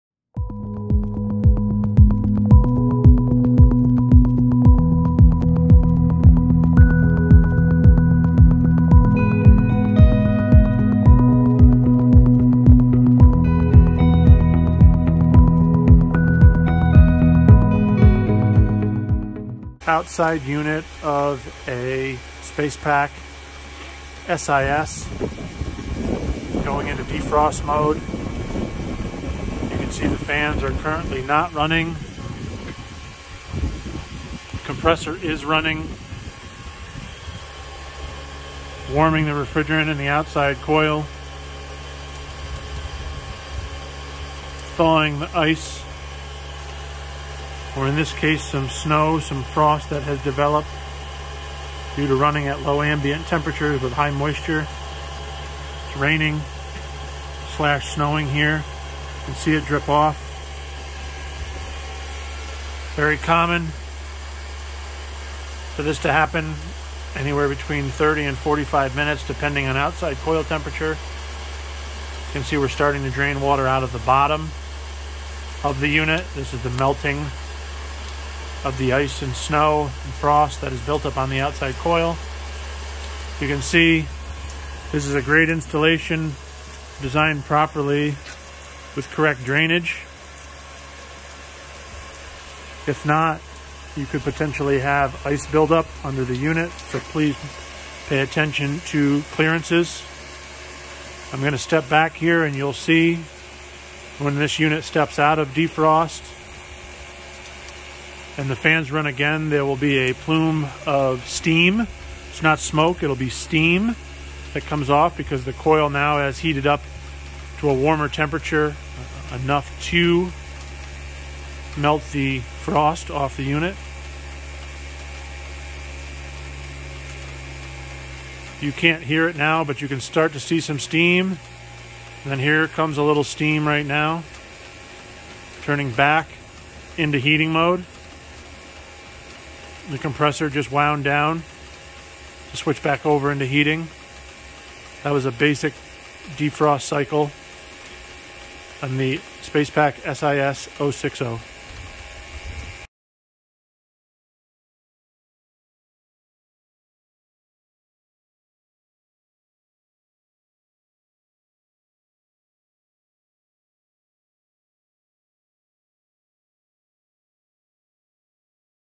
Cold Climate Split-System Air-to-Water Heat Pump in Defrost Mode
Solstice Inverter Split (SIS) air-to-water heat pump in defrost mode.

Application: (2) Solstice Inverter Split (SIS-060) air-to-water heat pumps supplying 120 degree water to a SpacePak 80-gallon buffer tank for whole-house radiant floor-heating.

Located in Killington, Vermont and filmed in November 2021 during installation.